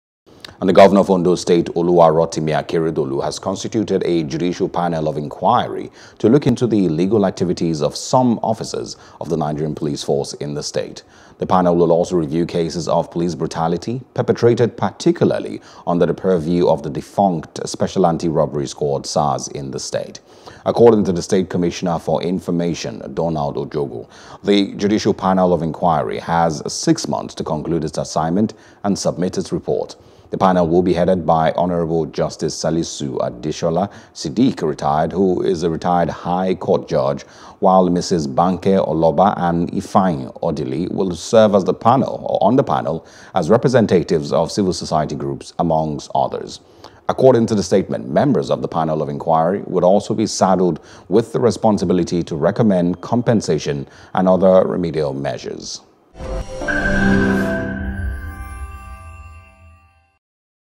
ENDSARS Protests: Akeredolu Constitutes Judicial Panel Of Inquiry (NEWS | NIGERIA)
ENDSARS Protests: Akeredolu Constitutes Judicial Panel Of Inquiry